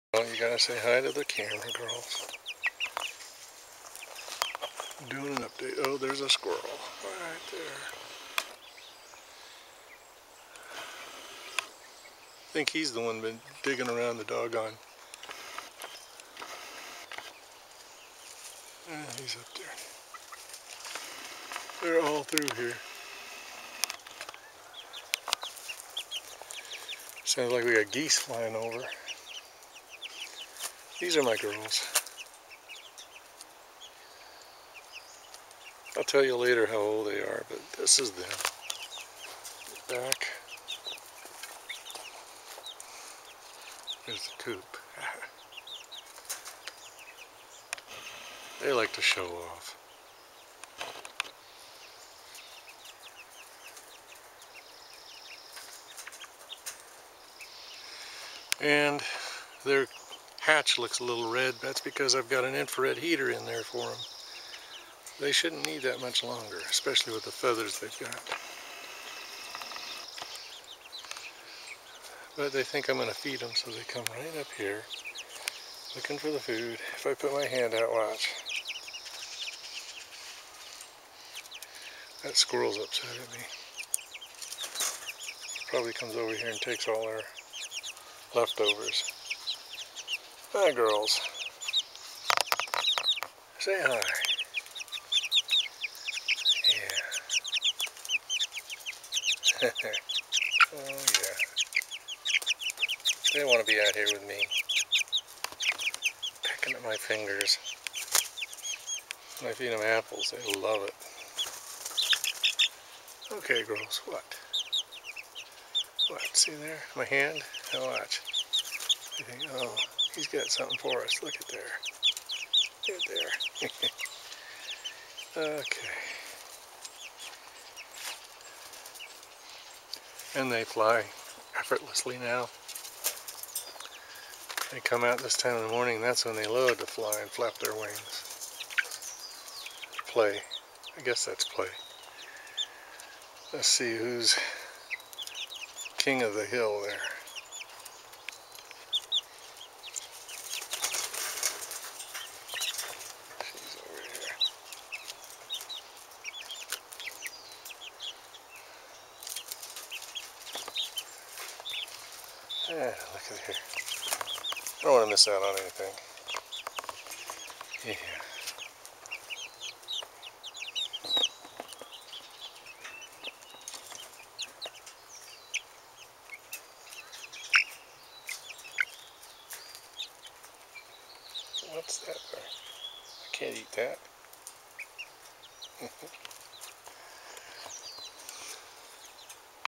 First Iteration Of My Chicken Coup
Fresh paint job, chicken wire chicken run and infrared lights.